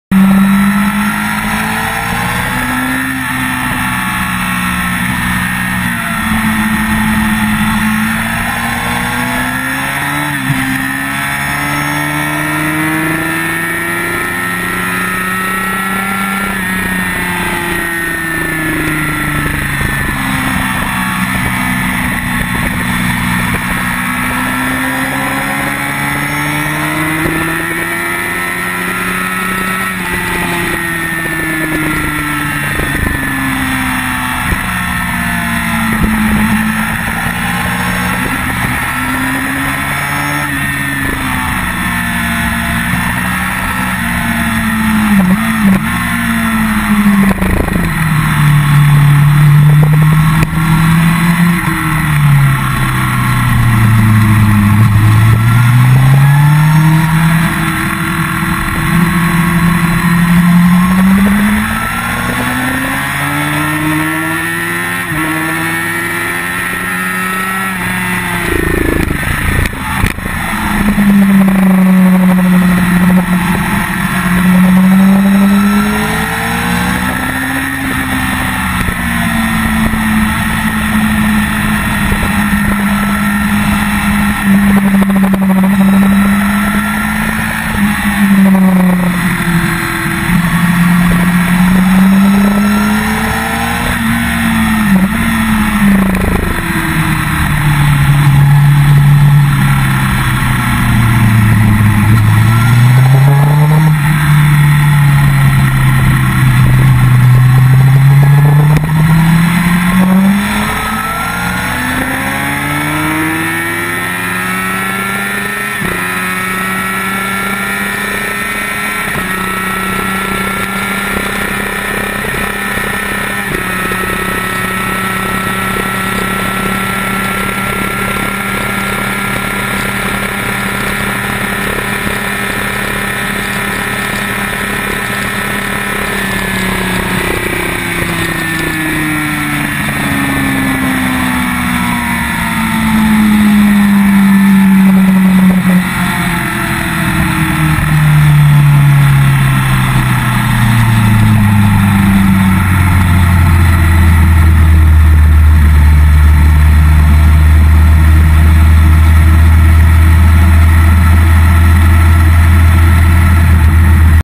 RAF Waddington Advanced 6
Free for all at the end of the day. Spot the Pillion from 1:40